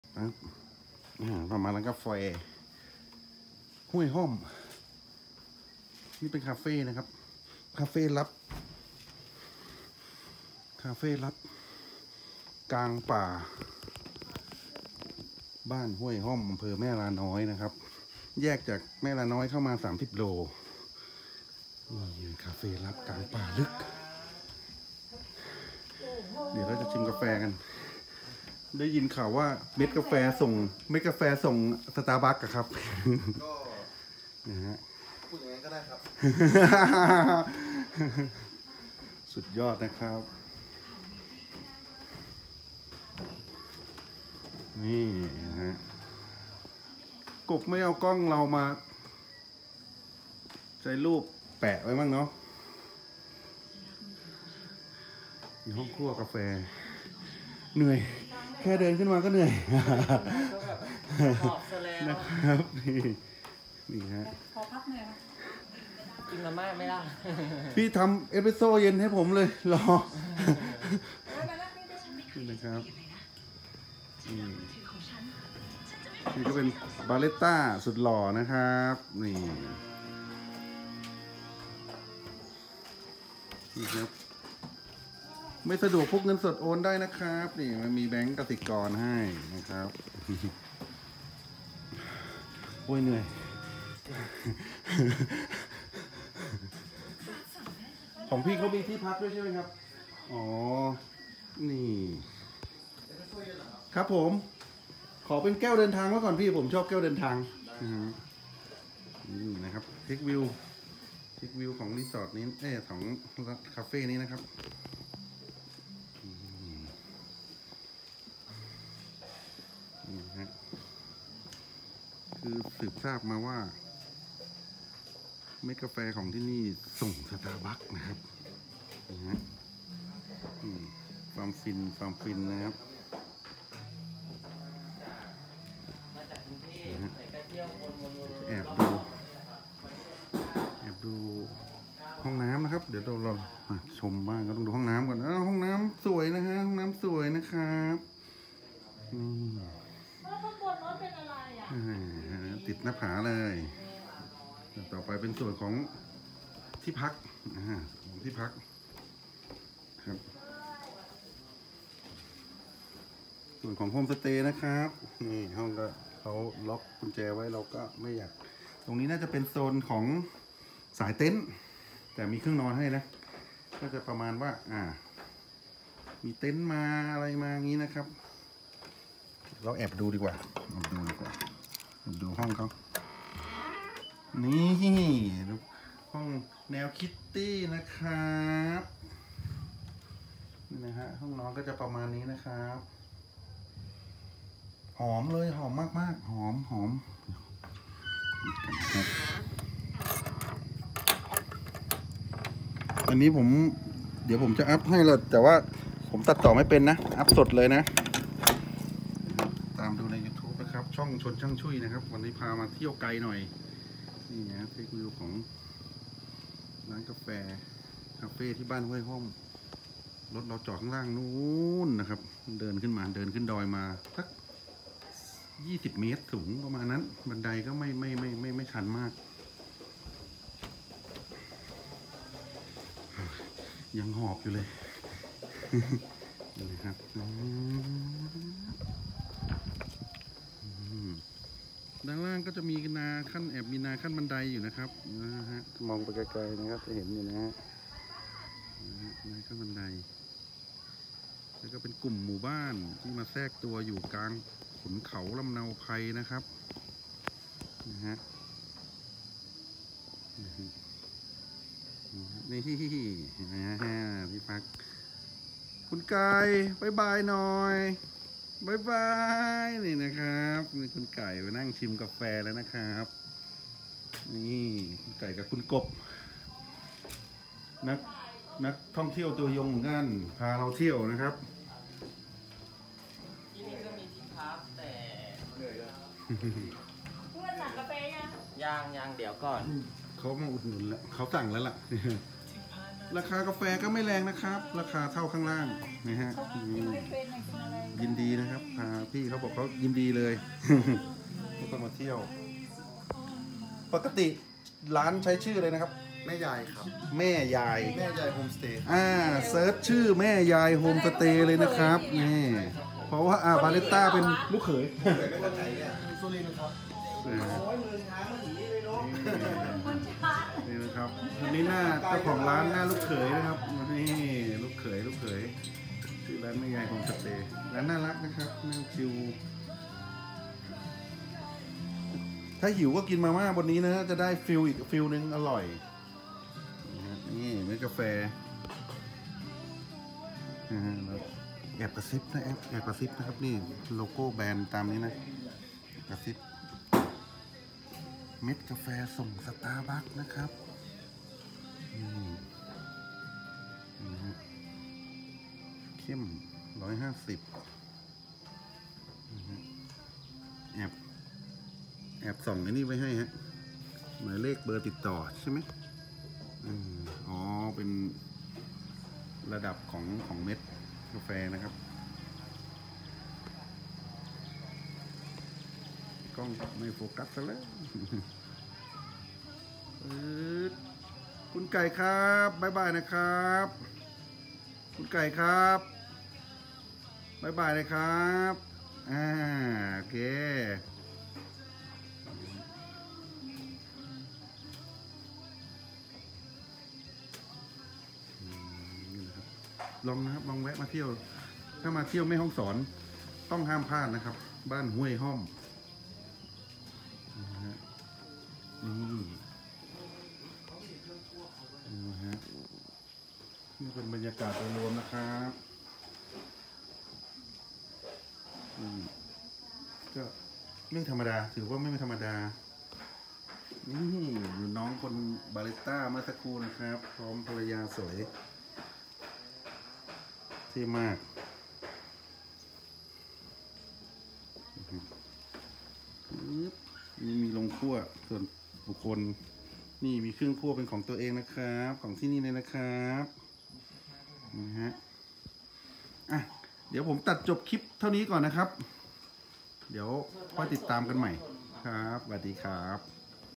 พาเที่ยว บ้านห้วยห้อม แม่ยาย โฮมสเตย์
@WeerachonAranyik  พาเที่ยวบ้านห้วยห้อมโฮมสเตย์อำเภอแม่ลาน้อยจังหวัดแม่ฮ่องสอนแหล่งผลิตเมล็ดกาแฟ สตาร์บัค